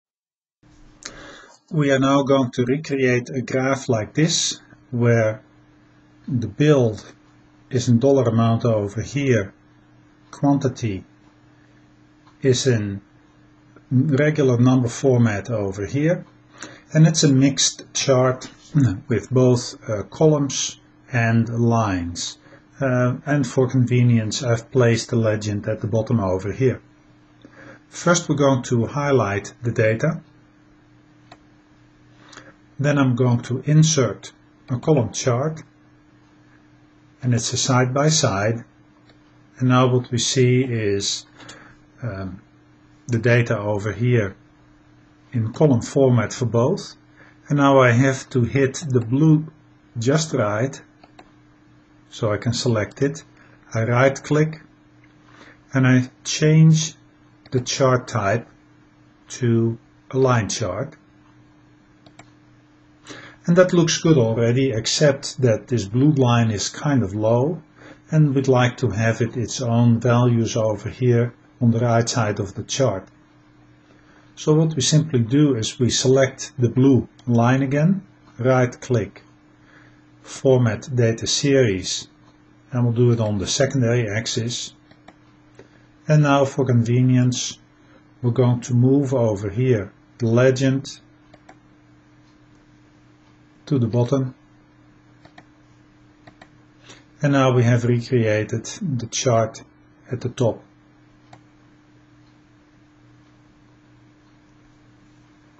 Excel_TwoVerticalAxes.wmv
Sometimes you want to display the values of a mixed chart on different axes. This video shows you how to add a second vertical axis on the right side of the graph.